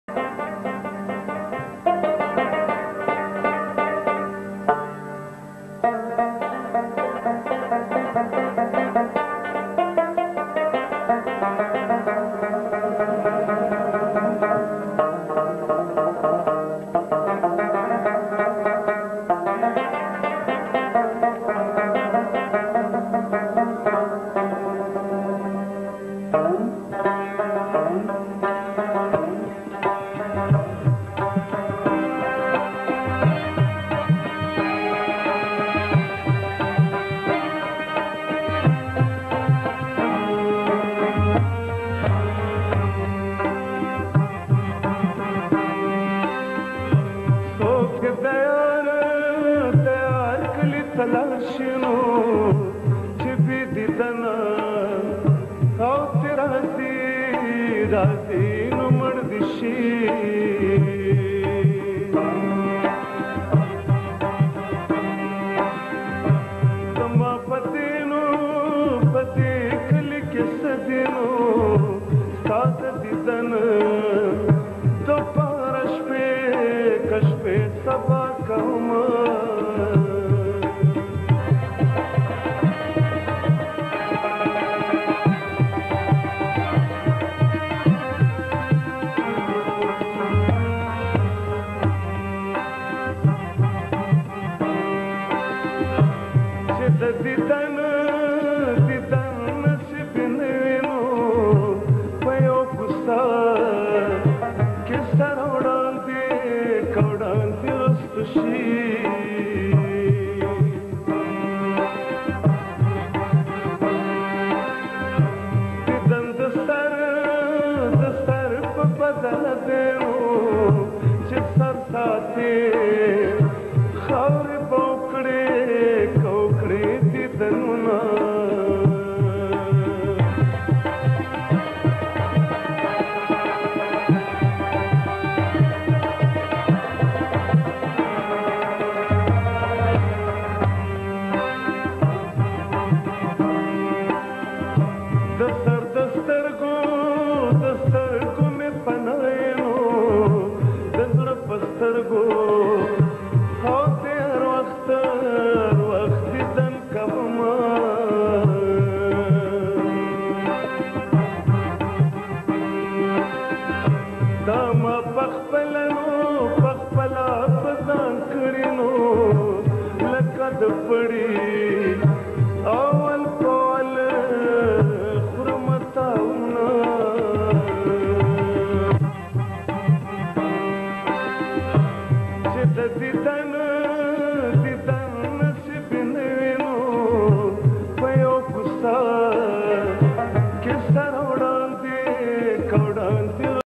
Khyal Mohammad | Tappey | خيال محمد | پښتو موسيقي | ټپې
A pashto folk song recorded at PTV Peshawar in 90s